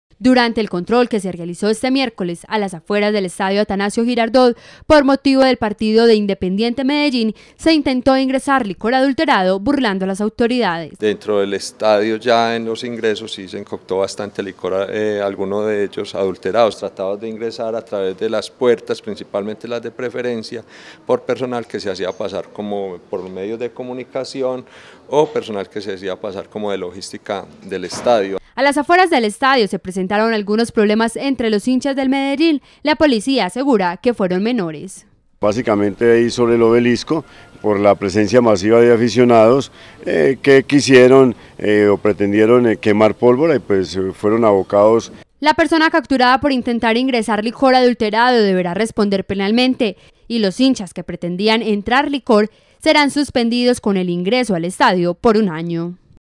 Intentaban Ingresar licor al Estadio en Medellín vs Santa Fe / Dic 18 2014 / Cosmovision Noticias
Intentaban Ingresar licor al Estadio en la Final Medellín vs Santa Fe: 38 personas fueron capturadas al pretender ingresar al estadio 168 medias de botella de licor adulterado. Lo que llama la atención de las autoridades es que usaban prendas o distintivos de medios de comunicación y de personal logístico para burlar a los anillos de seguridad.